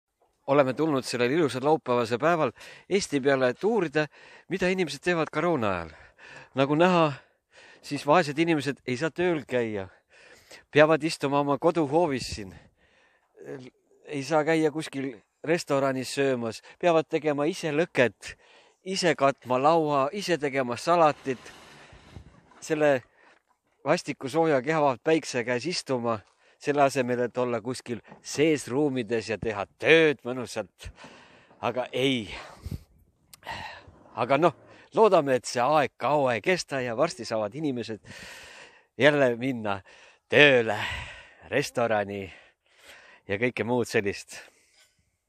UURIME MIDA TEEB EESTI RAHVAS KOROONA VIIRUSE GARANTIINIS
Reporter Miguel "Ehitame TV" saatejuht uurib mida teeb eesti rahvas kui kuhugi pole minba